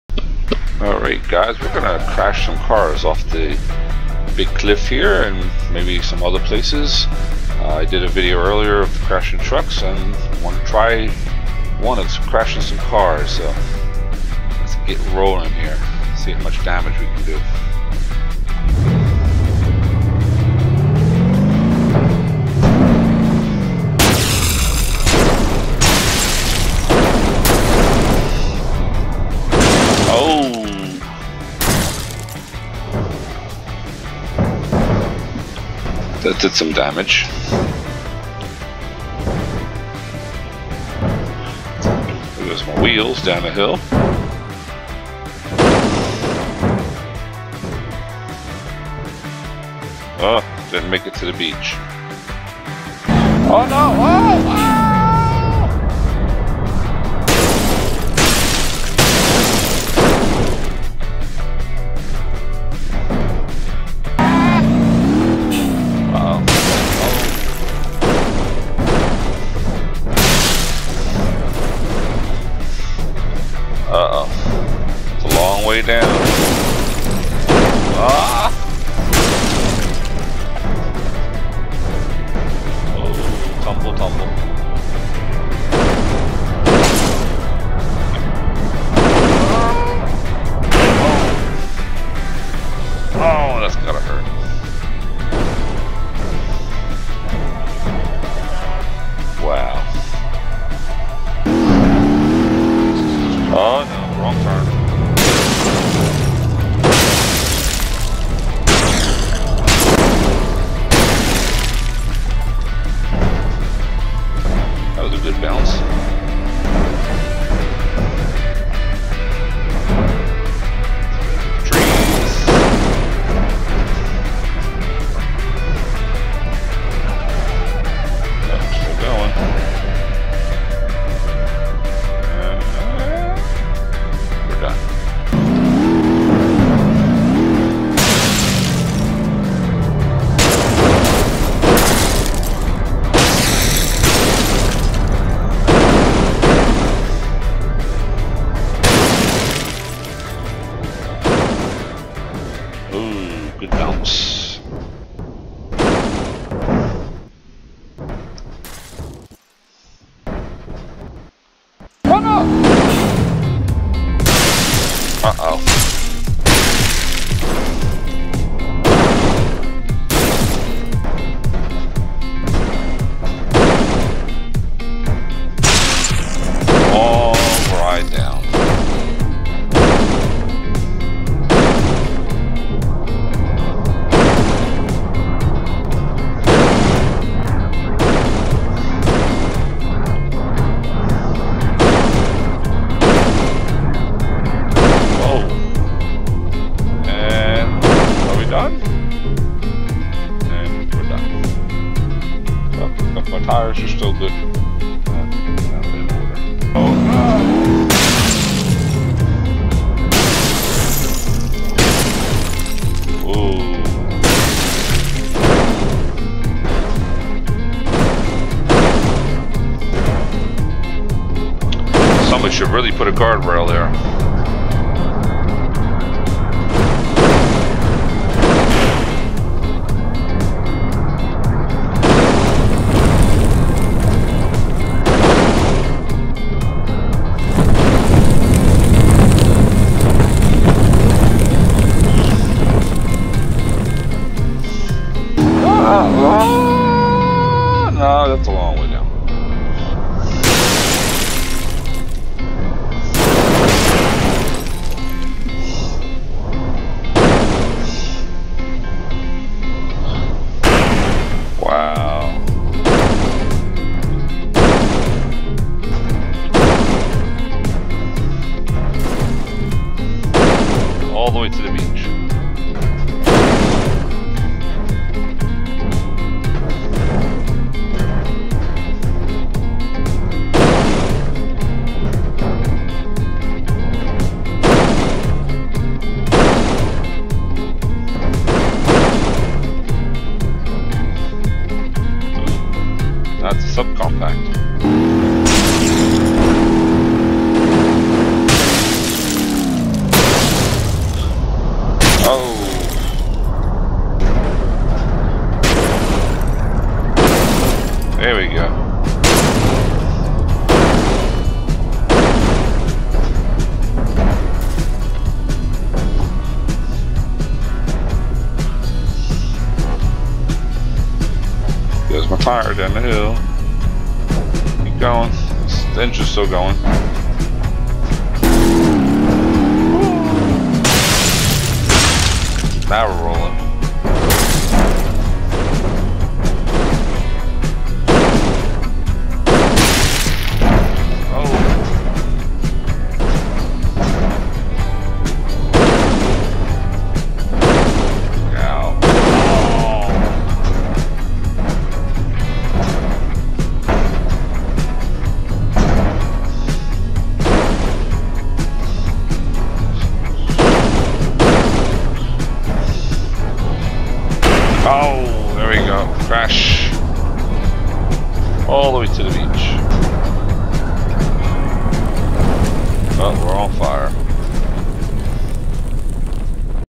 6 Minute Crash Video
part of a 6 minute video series. anything from car crashes, police chases, and battles.
Music licensed by Filmora